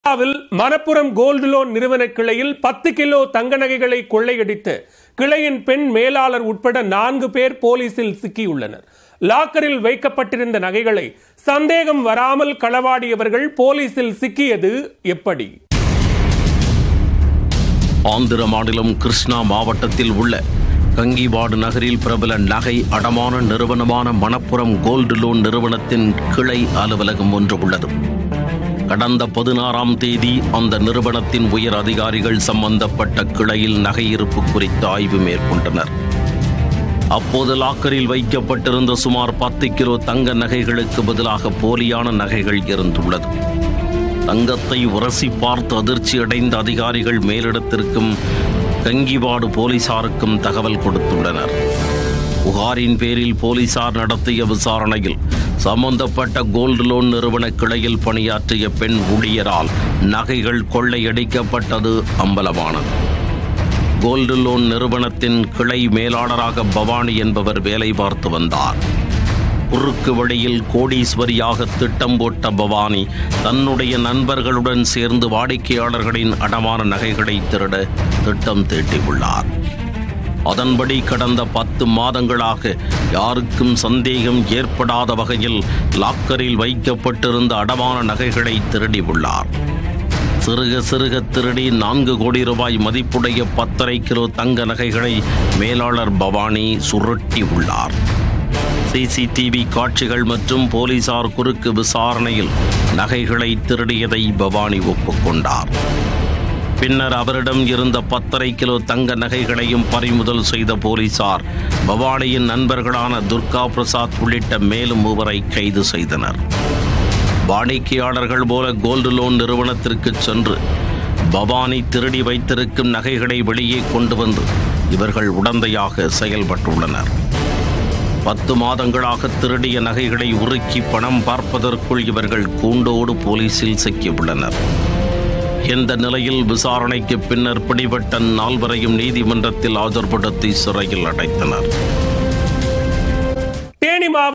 கோல்டு லோன் நிறுவனத்தில் கைவரிசை.. 10 கிலோ தங்க நகைகள் திருட்டு | Crime Time | Andhra Pradesh
ஆந்திராவில், மணப்புரம் கோல்டு லோன் நிறுவன கிளையில் 10 கிலோ தங்க நகைகளை கொள்ளை அடித்த கிளையின் பெண் மேலாளர் உட்பட நான்கு பேர் போலீசில் சிக்கி உள்ளனர். லாக்கரில் வைக்கப்பட்டிருந்த நகைகளை சந்தேகம் வராமல் களவாடியவர்கள்,  போலீசில் சிக்கியது எப்படி?